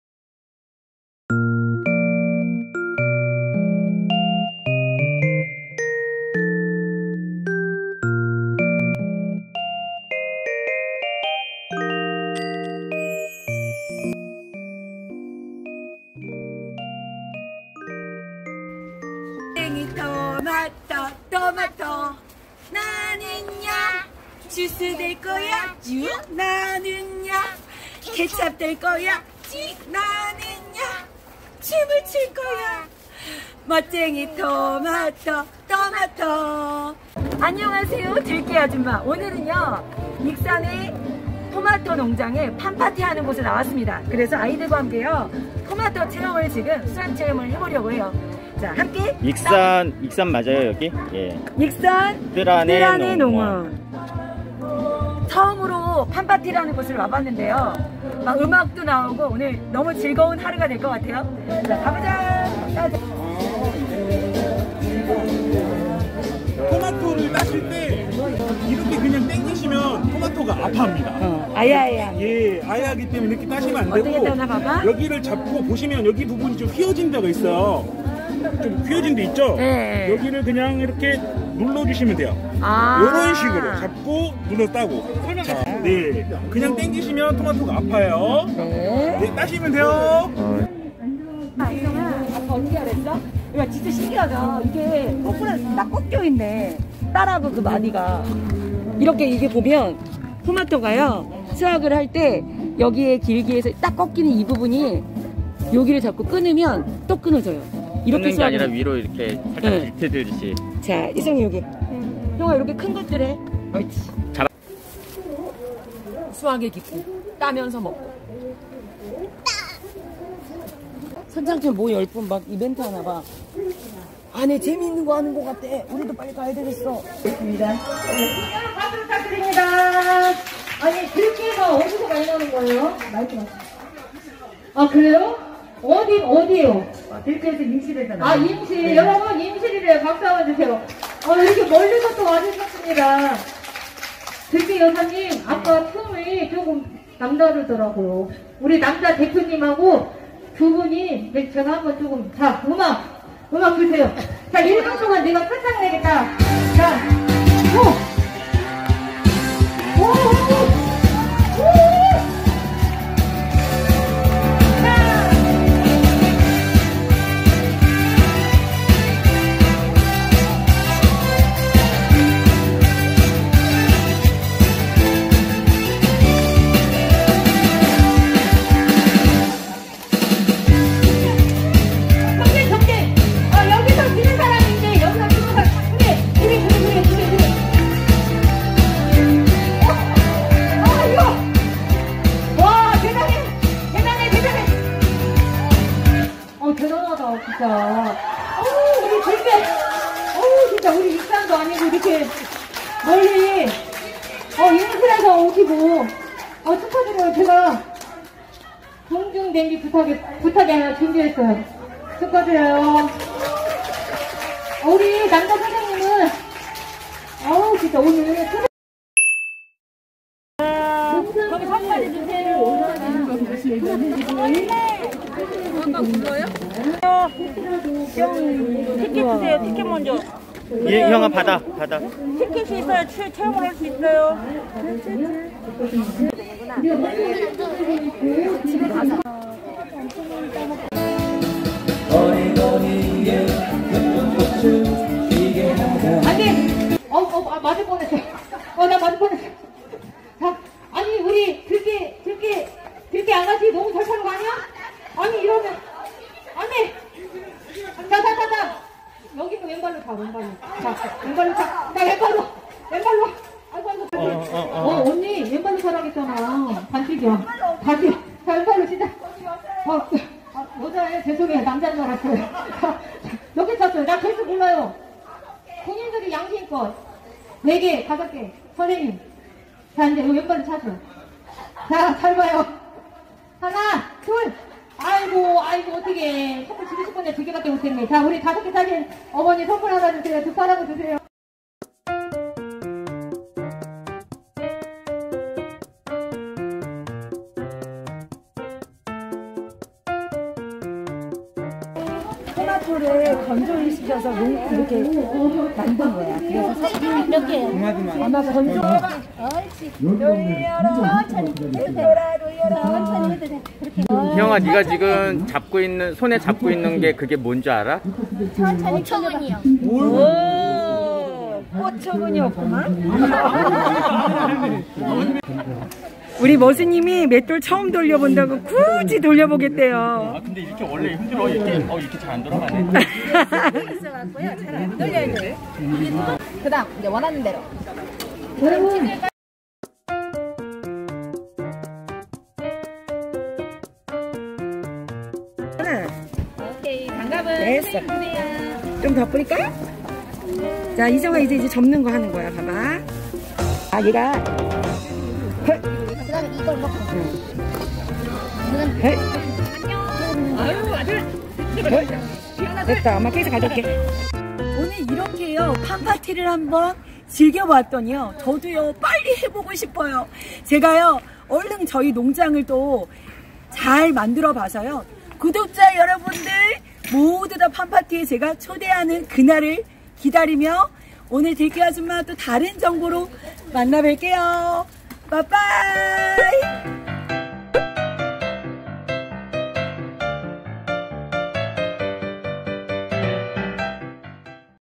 농장 팜파티에서도 들깨아줌마 춤한판 추고 왔어요ㅋㅋ
전북 익산 뜰안에 농장
팜파티에 다녀왔습니다.
여기서도 들깨아줌마의 춤바람은 계속 된답니다. ㅋㅋ
저희 농장도 들깨가족 구독자 분들과 함께
언능 팜파티 하는 그날을 기다려봅니다.

♡행복한 농사꾼이 만든
생들기름&들깨가루&건고사리 문의는...
들깨아줌마에게 띠로링~